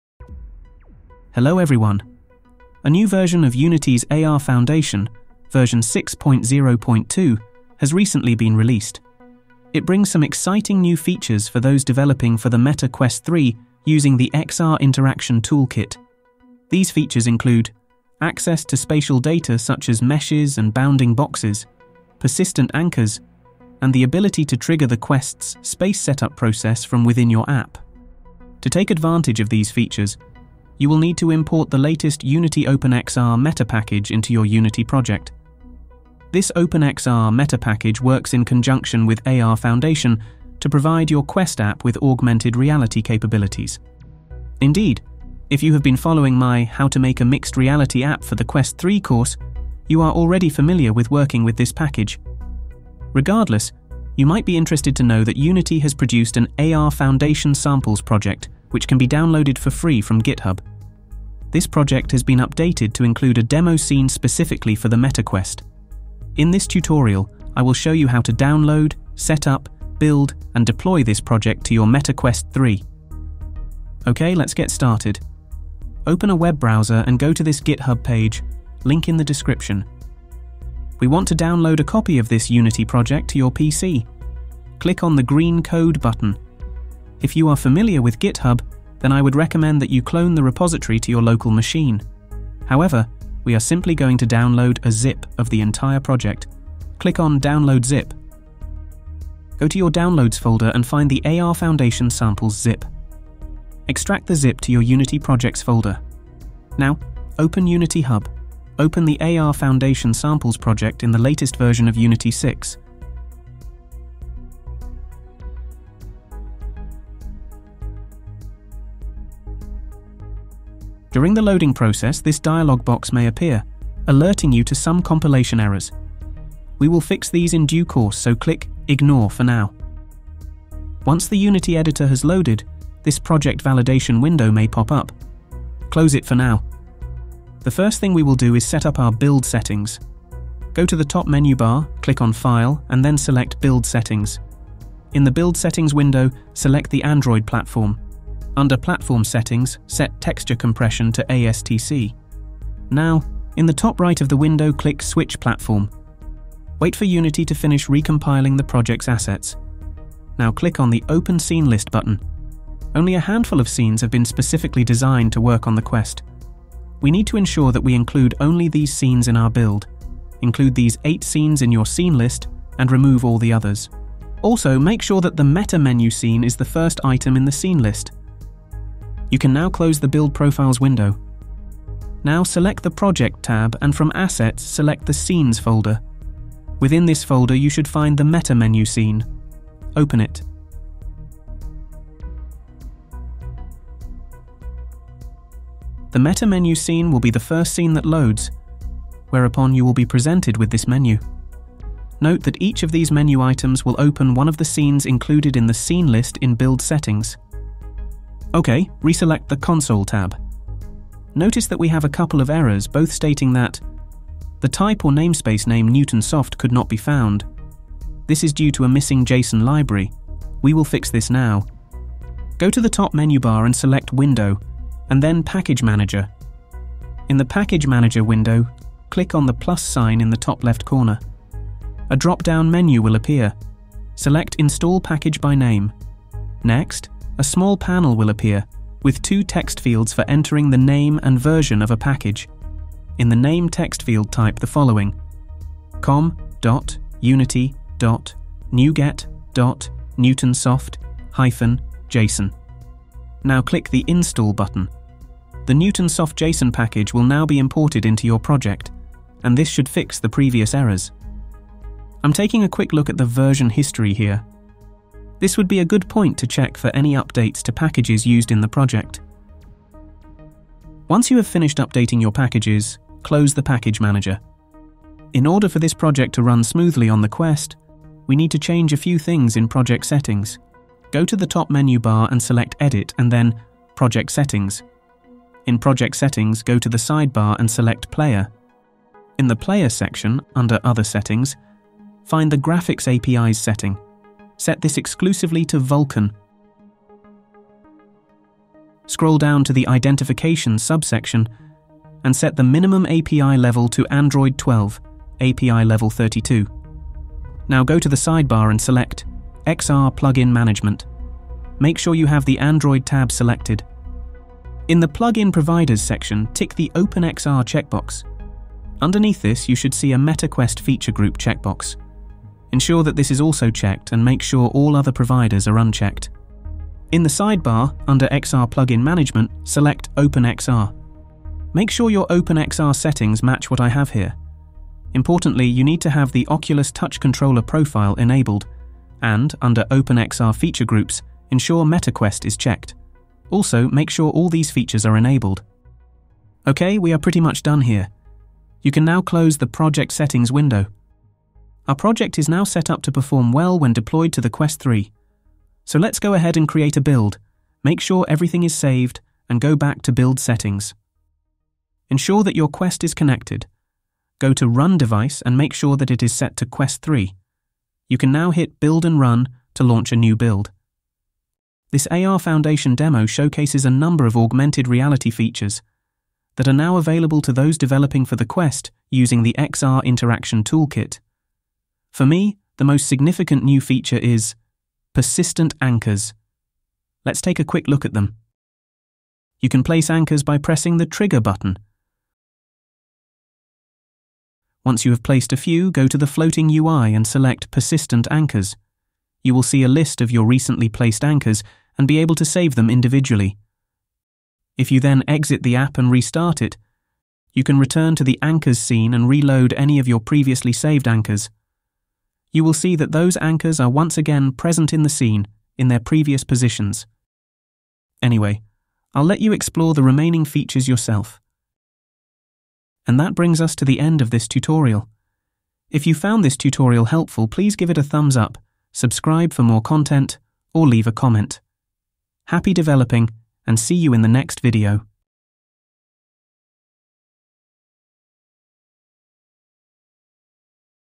Get 'AR Foundation Samples' Running on Meta Quest 3
Learn how to get Unity's 'AR Foundation Samples' project running on your Meta Quest 3. This tutorial covers configuring build and project settings, then deploying the project to your Quest 3.

▬ Timestamps ▬▬▬▬▬▬▬▬▬▬▬▬▬▬▬▬▬

0:00     - Intro
1:13     - Download Project
1:46     - Open Project In Unity
2:10     - Build Settings
3:01     - Open Menu Scene
3:29     - Import Newtonsoft Json
4:35     - Project Settings
5:51     - Build And Run
7:02     - Outro

▬ Useful Links ▬▬▬▬▬▬▬▬▬▬▬▬▬▬▬▬▬

► Newtonsoft Json - Package Name:
com.unity.nuget.newtonsoft-json

▬ Credits ▬▬▬▬▬▬▬▬▬▬▬▬▬▬▬▬▬▬▬